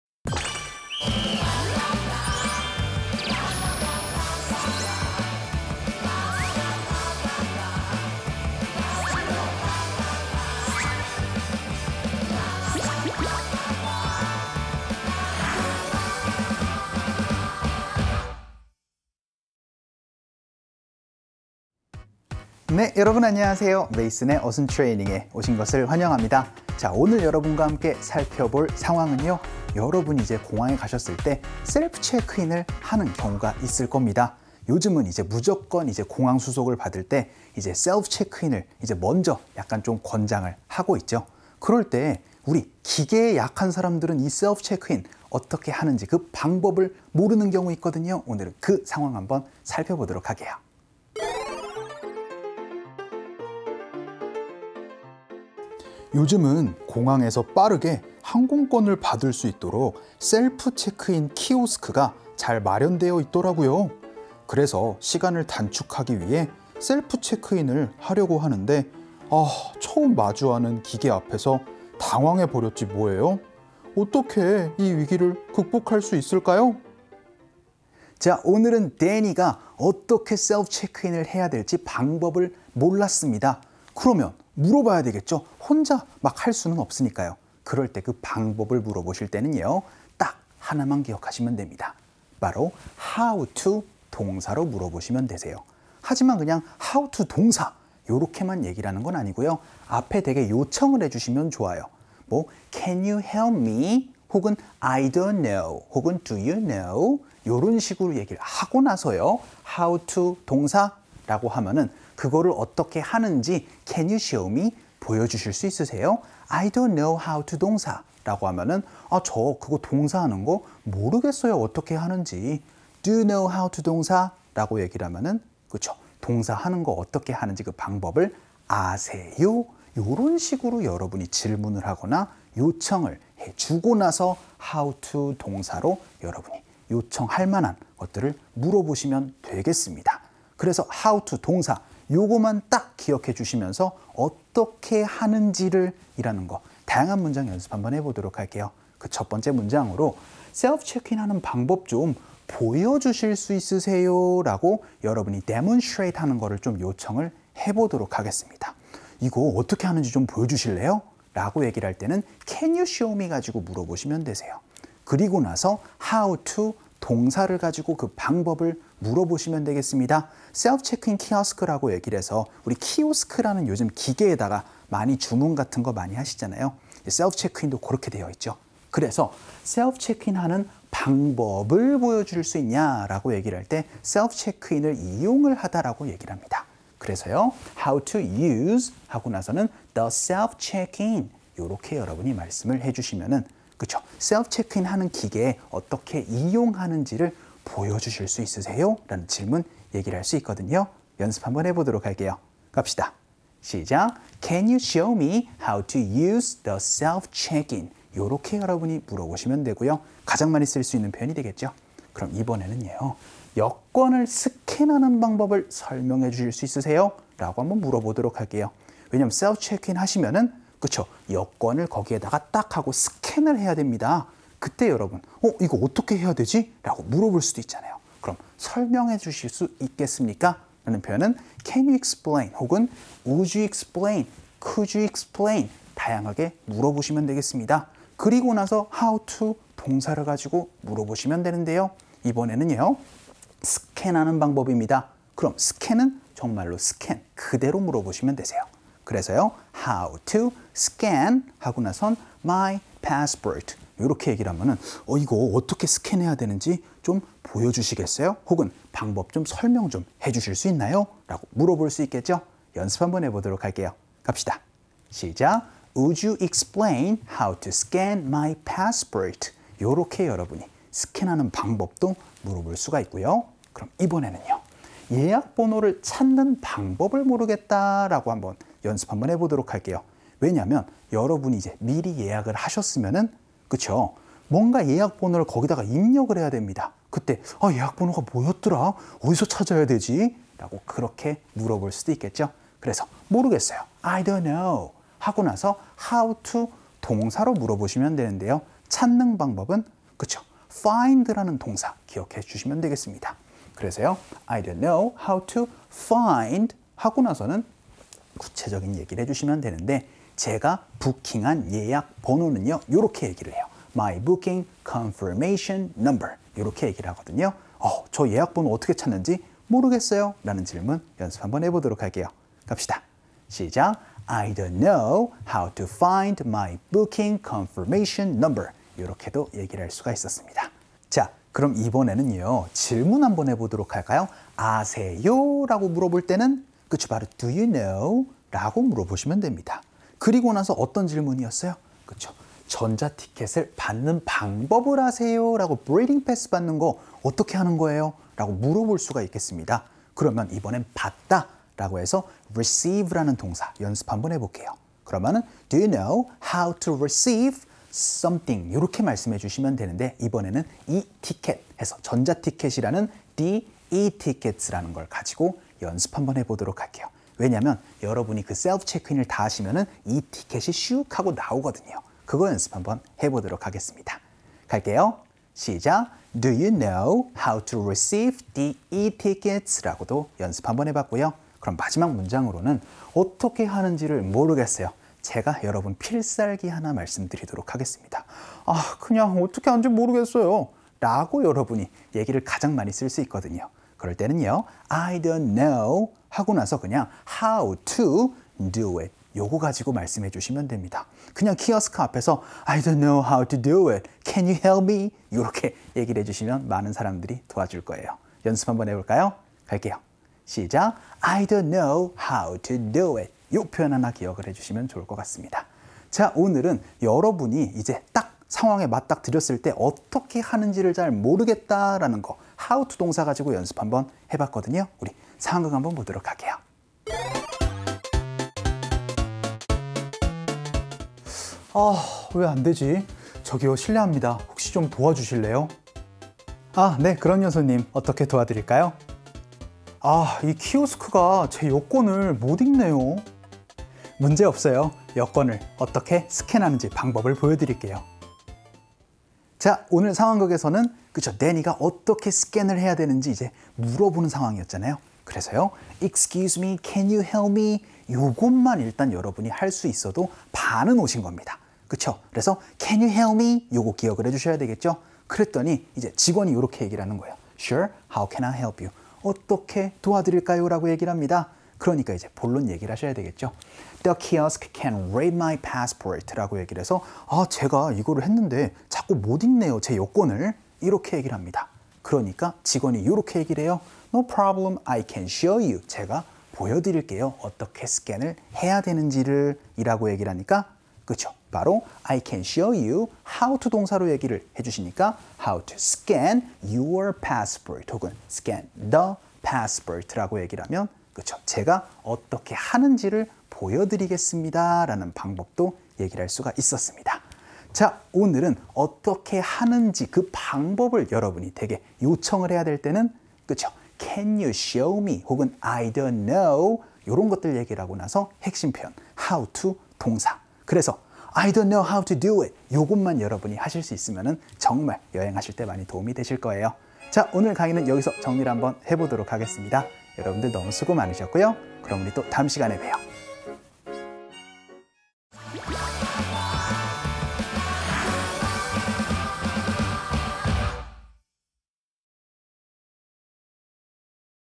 매일 10분 영어 - 메이슨의 어순 트레이닝 - Unit 916. 셀프 체크인 문의
영어로 길게 말해도 문장이 꼬이지 않고 자유롭게 회화를 구사할 수 있도록 어순 구조와 감각을 훈련시키는 프로그램.